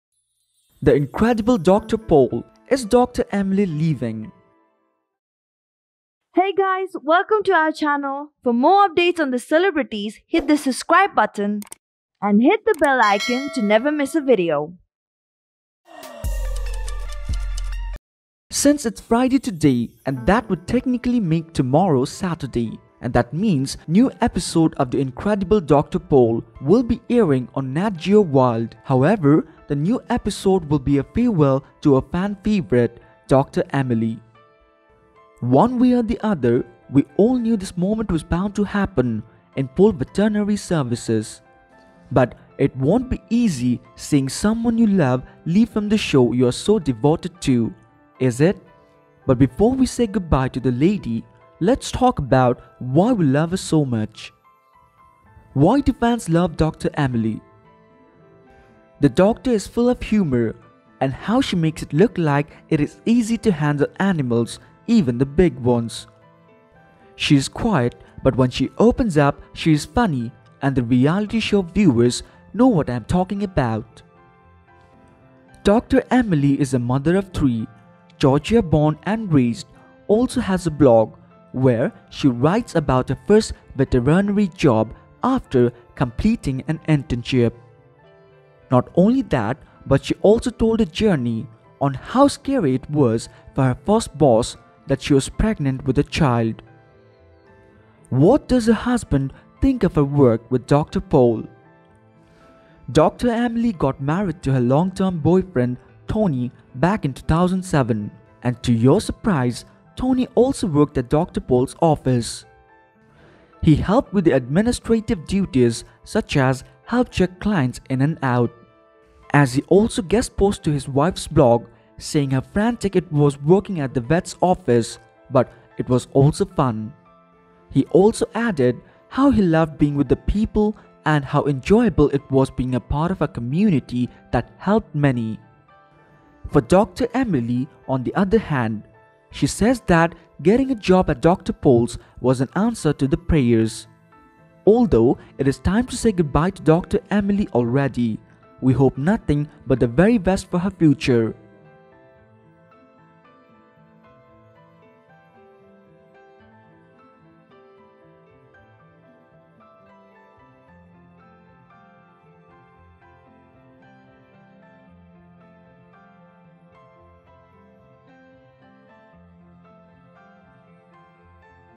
Final Goodbye to Dr Emily Thomas on Dr Pol; Is She Leaving forever?
The Incredible Dr. Pol: Sadly the NatGeo's reality show, Incredible Dr. Pol viewers will be saying goodbye to one of their favorite vet on the show. As the new episode that is about to air on Saturday night will be a farewell to Dr. Emily Thomas. But will she be coming back to the show again? Or is this really the final episode for her?